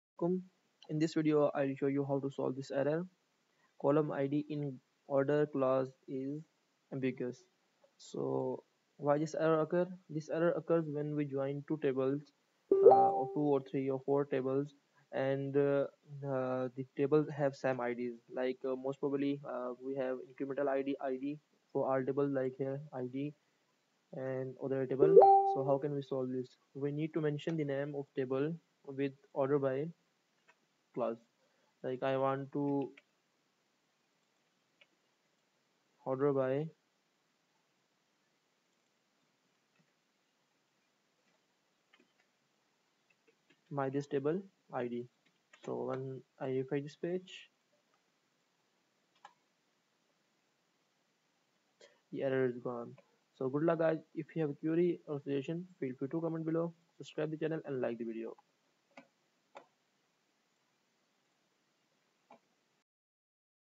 Column 'id' in order clause is ambiguous codeigniter
in this video, I will show you how to solve this error Column 'id' in order clause is ambiguous :

The error means that you are trying to order by a column name that is used in more than one table. Update your order_by statement with the name of the table that has the column you want to order.